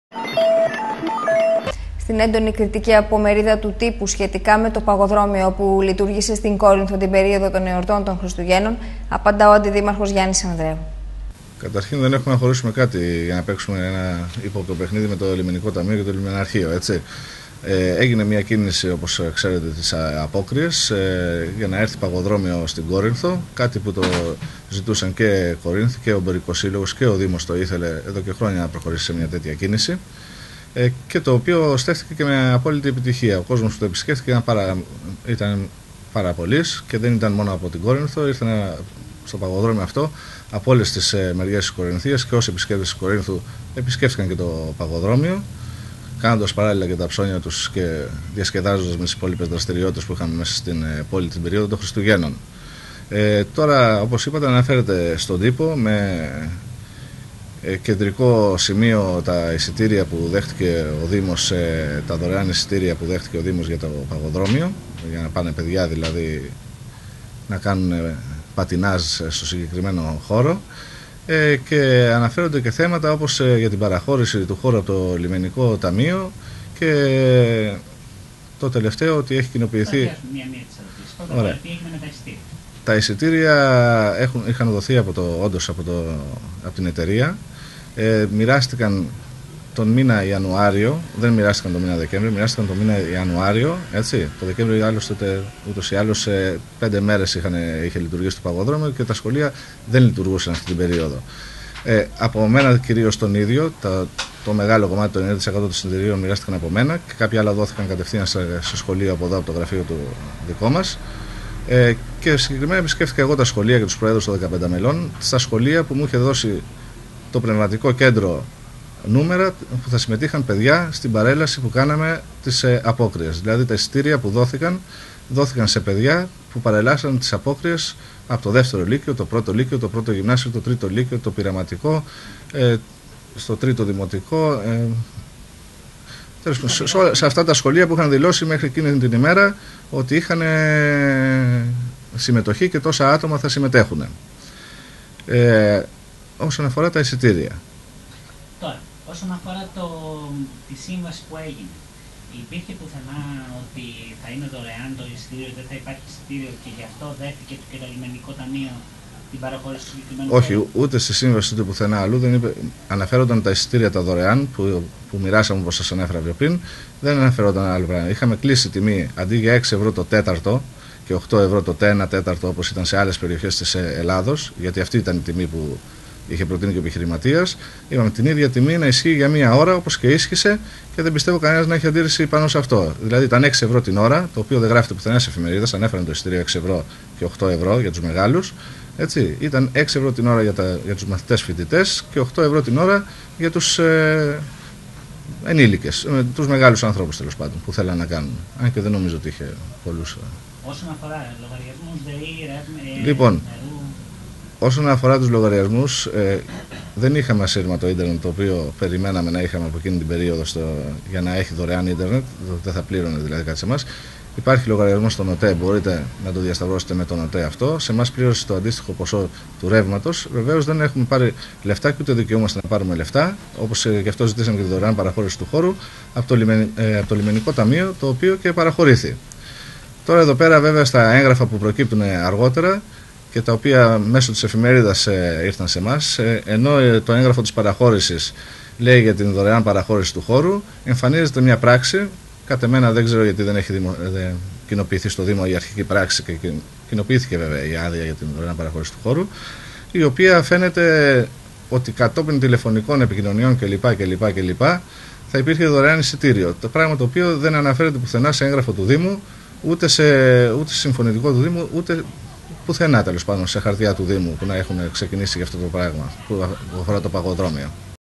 ΚΟΡΙΝΘΟΣ 21 3 2010 DRTV
Κόρινθος - Ο αντιδήμαρχος Γ.Ανδρέου για το παγοδρόμιο.